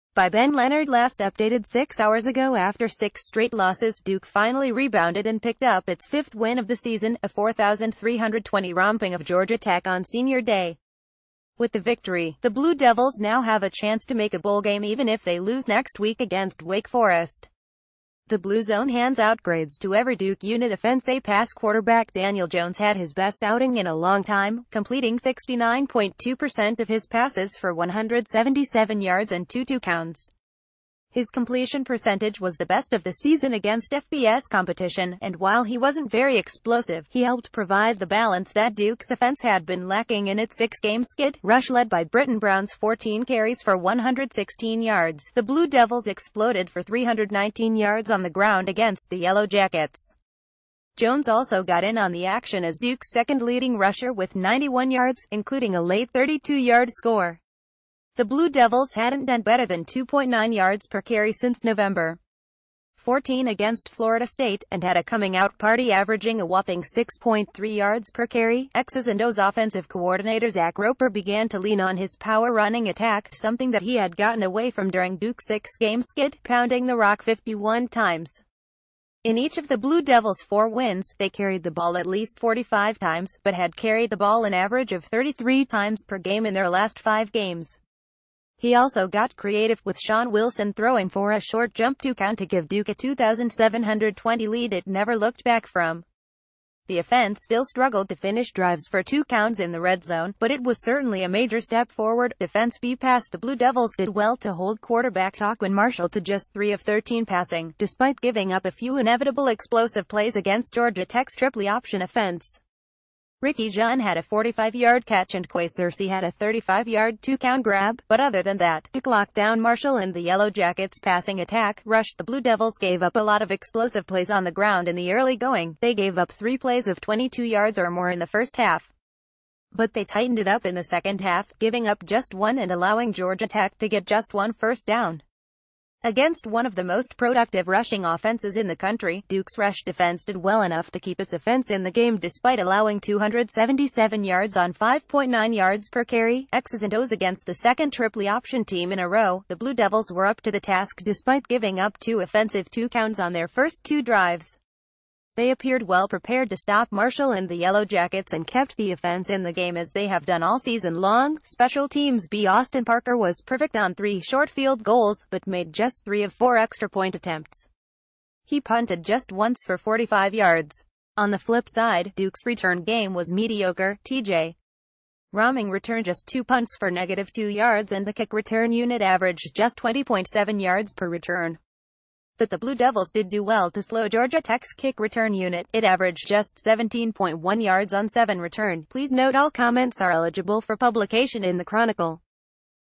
Sport News - Making the grade: Duke picked up its first win in seven tries
Sport News - Sport News - Making the grade: Duke picked up its first win in seven tries

By Ben Leonard Last updated 6 hours ago After six straight losses, Duke finally rebounded and picked up its fifth win of the season, a 4320 romping of Georgia Tech on Senior Day. With the victory, the Blue Devils now have a chance to make a bowl game even if they lose next week against Wake Forest. The Blue Zone hands out grades to every Duke unit  Offense A Pass Quarterback Daniel Jones had his best outing in a long time, completing 69.2 percent of his passes for 177 yards and two toucowns. His...